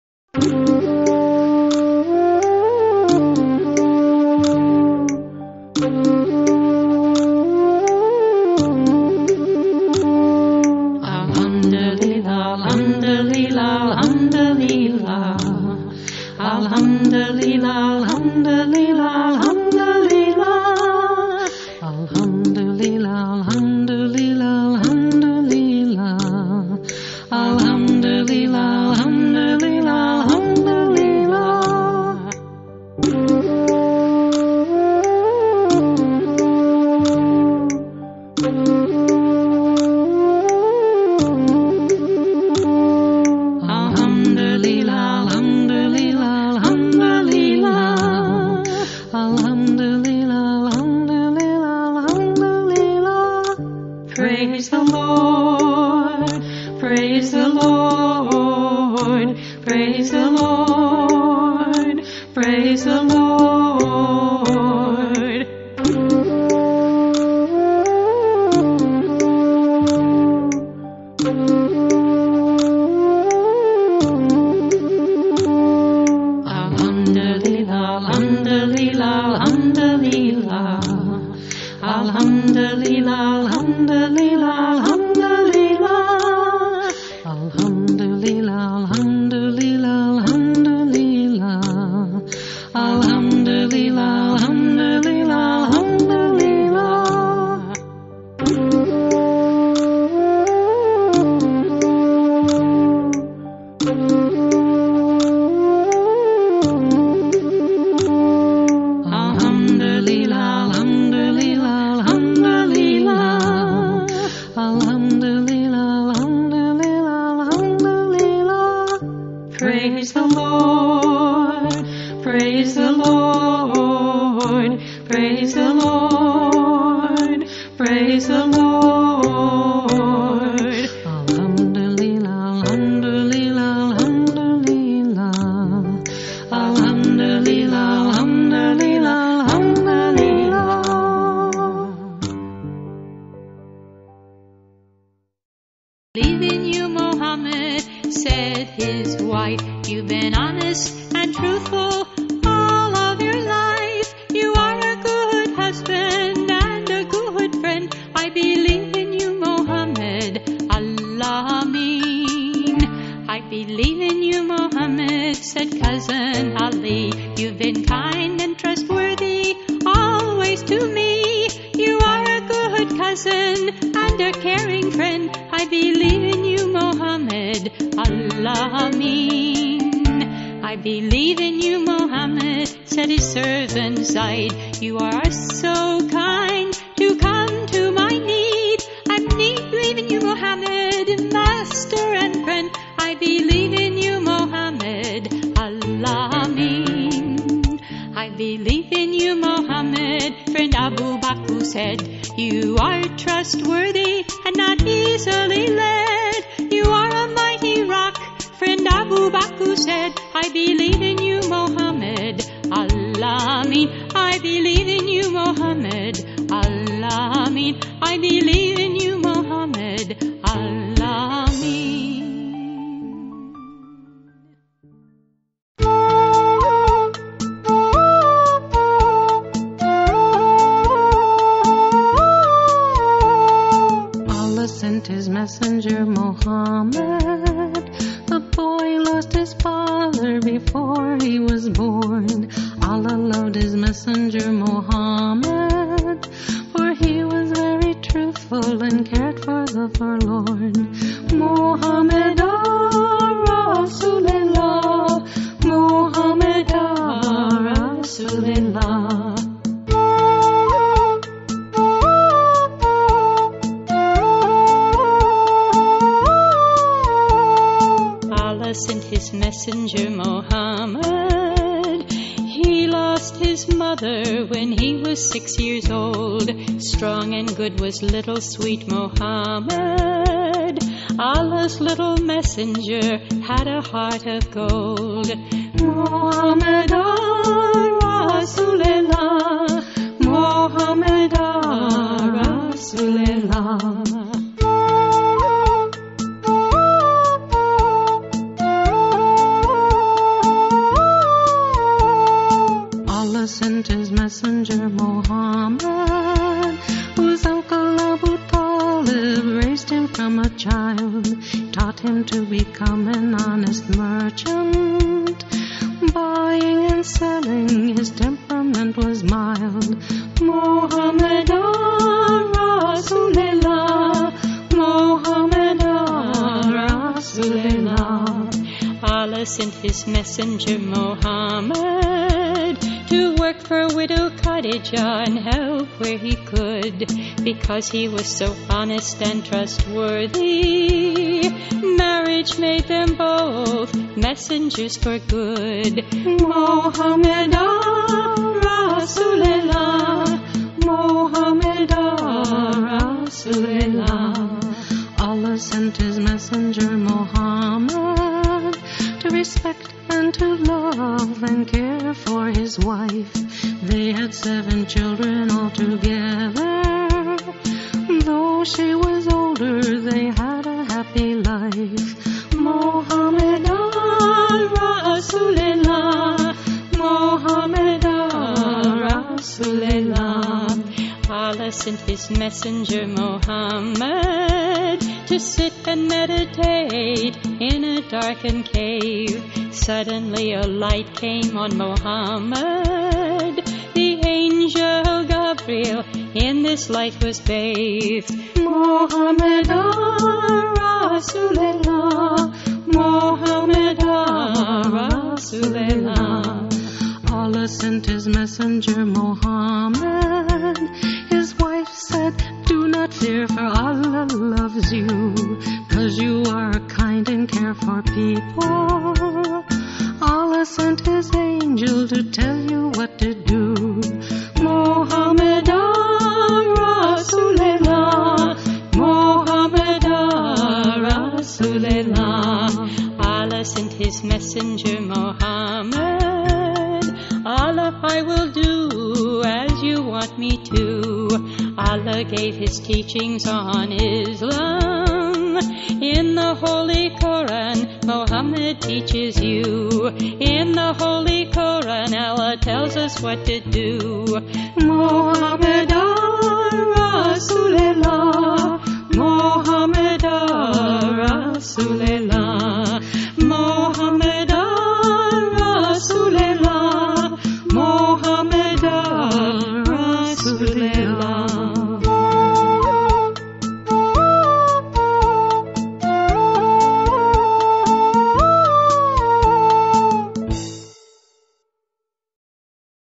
The Children's Book of Islam - Part 1
I wrote several books for children, on some of the world's religions. This is the first one I turned into a video, with me singing several songs to go along with it. Enjoy!